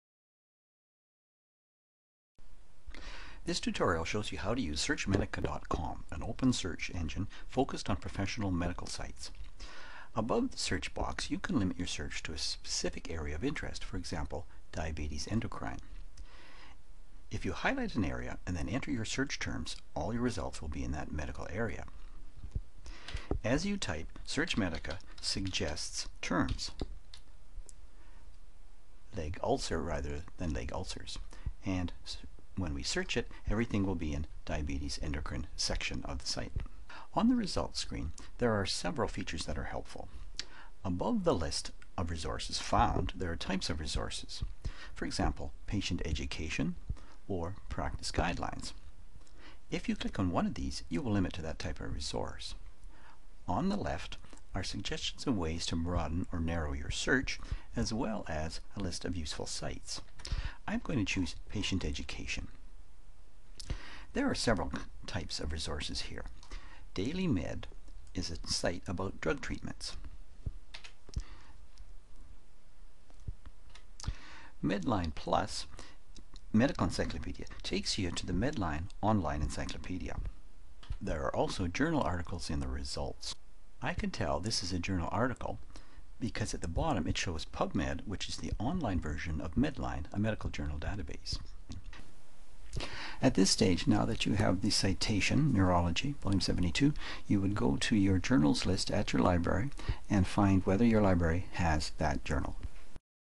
SearchMedica.com guide
Demonstrates how to search the SearchMedica.com website. From the section of the search engine's website: "With guidance from our advisory board of specialty physicians and our staff editors, SearchMedica scans well-known, credible journals, systematic reviews, and evidence-based articles that are written and edited for clinicians practicing in primary care and all major specialties. Using similar expertise, SearchMedica also selects and scans patient-directed websites, online CME courses, and government databases of clinical trials and practice guidelines."This tutorial was originally created by Richard Baer and Catherine Winter (Camosun College) on Oct 29, 2009 using Camtasia Studio 6.The Camtasia Studio 6 source files and original .mp4 file for this tutorial can be downloaded from the ANTS collection at the University of Calgary Dspace reporsitory: